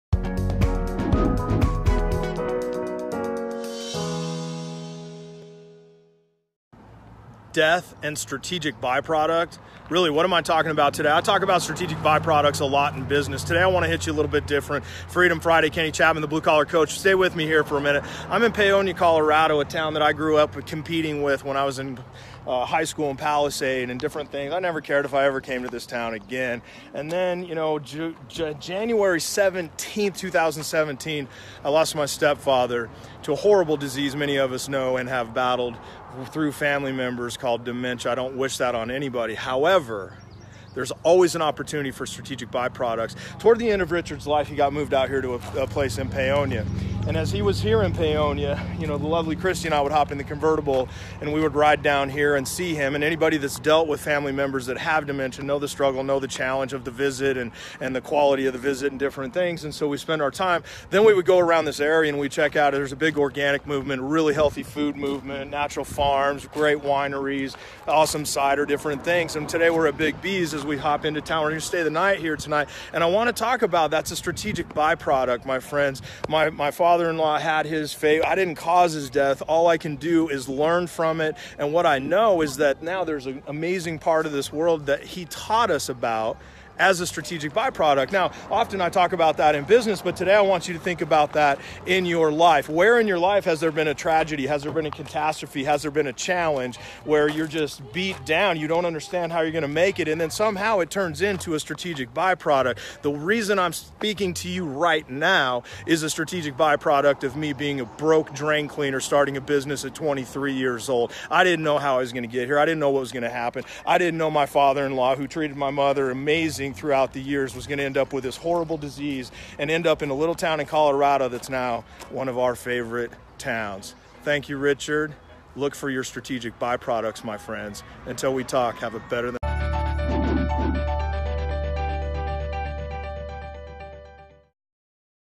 Death and Strategic Byproducts {Freedom Friday Focus}
My step father Richard passed away January 17, 2017 after a long and intense battle with Dementia. One of the most destructive diseases I’ve been around and certainly don’t wish that experience on anyone.

That being said, life has an interesting way of throwing you challenge after challenge as you are tested to learn all you can, become all you can, and contribute all you can. Many people say without much emotion things like, “Everything happens for a reason”.  However, you know immediately based on their vocabulary and outlook if this is a true deep-seated belief or they are trying to convince themselves of this fact.

My mentor, Dan Sullivan, taught me the concept of Strategic Byproducts in business and in life. To this point, I’ve shared a lot of these and taught our clients how powerful these can be in business. I’ve primarily kept my focus on business, but today, I’m going a bit beyond our professional lives.

A Strategic Byproduct is something that happens to you because of an action you took for a different reason. Goals lead to goals. Life leads to life. And resistance leads to more resistance. We know change is the only permanent thing in life, but we resist it so much and change is where Strategic Byproducts are born.

The Blue Collar Success Group, Inc. is simply a Strategic Byproduct of me launching a little dead broke drain business, scared to death with no skill or direction at 23 years old. Sometimes we don’t connect dots like this throughout or lives.

Back to Richard, he battled until his final day in an Alzheimer’s Care Facility in Paonia, CO. I grew up in Palisade and Paonia was a die-hard sports rival, so I never liked the town, thought anything about the town, or ever cared if I saw it again after my final game senior year.

Once Richard was placed in Paonia for care, The Lovely Christy and I would take convertible rides for the 90-minute drive down for visitation and began to checkout the West Elks wine region, local culture, and cuisine. Tiny little town. Unbelievably awesome! So cute, so cultured!

Though Richard is gone and his memory lives on, we are grateful and enjoy the Strategic Byproduct he gave us by showing us this lovely little gem of a Colorado town. We’ll be staying at a little B&B in Paonia tonight.

What about you? Death. Tragedy. Challenge. Strategic Byproduct? Could depend on your perspective.